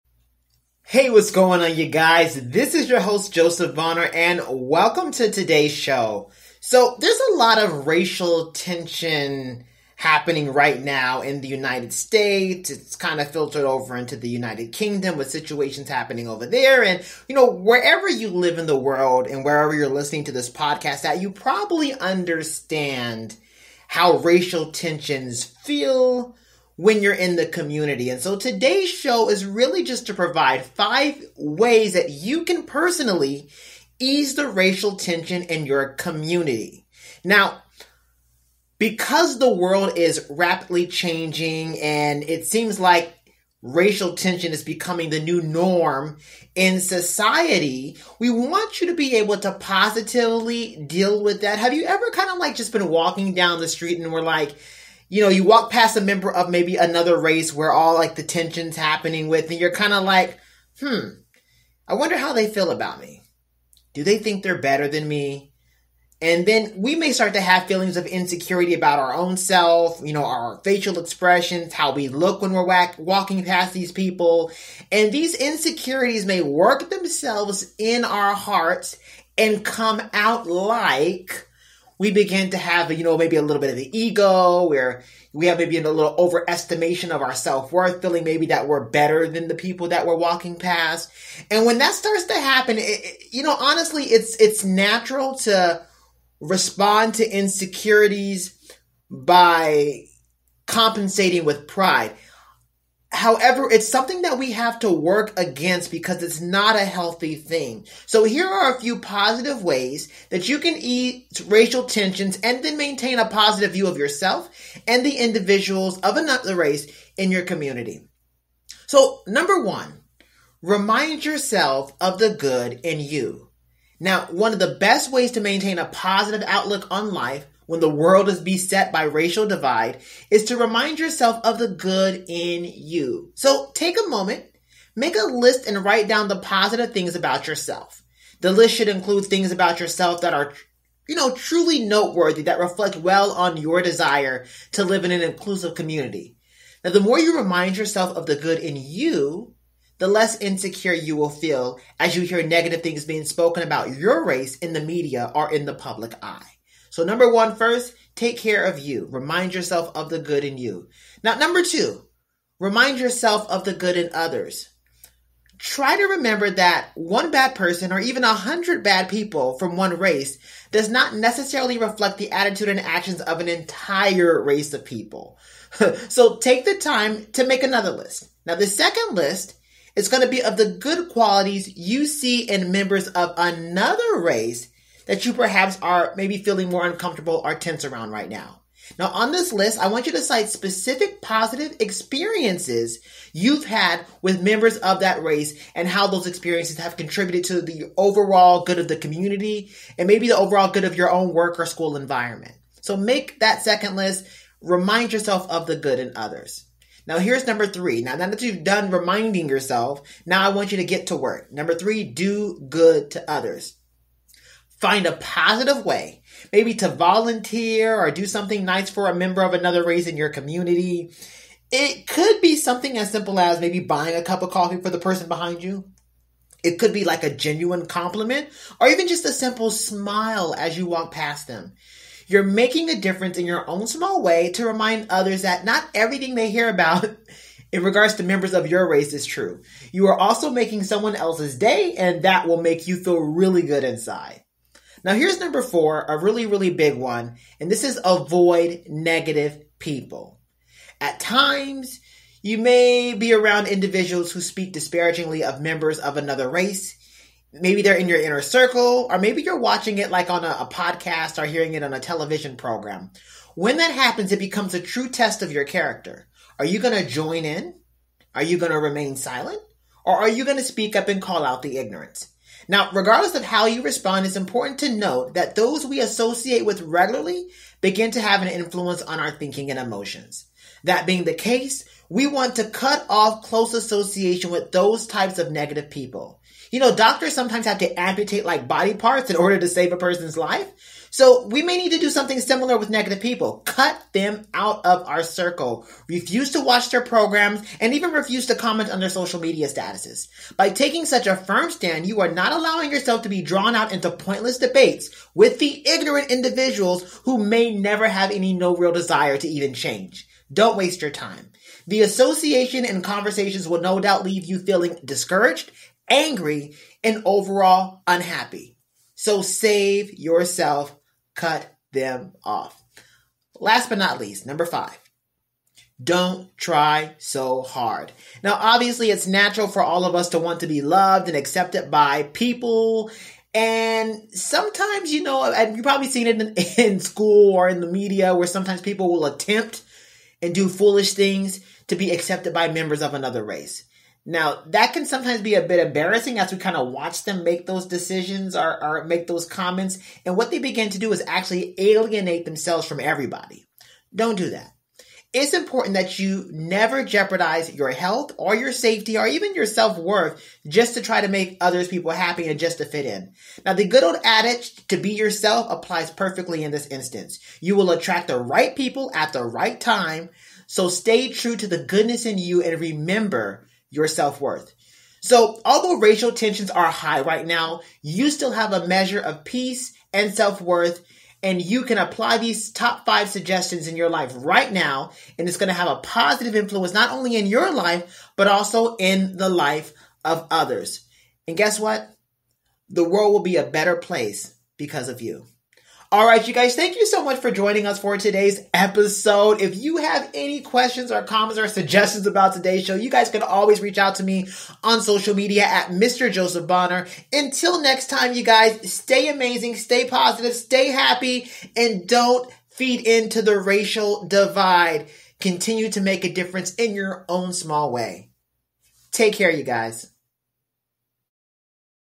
5 Ways To Ease Racial Tensions In Your Community
When racial tension is at an all time high your part of the world, even the most simplest task can seem daunting. As you walk past people of other nationalities, your thoughts and feelings may sometimes get the best of you. You may begin to ask yourself questions; Do they look at me differently? Do they think I’m racist? Do they think they are better than me? As a result, you may try to compensate for your feelings of insecurity by walking around with a bit of an ego or even an unhealthy overestimation of self-worth, feeling as if you are better than “They” are.
Here are a few positive ways you can ease racial tension and maintain a positive view of yourself and individual members of another race.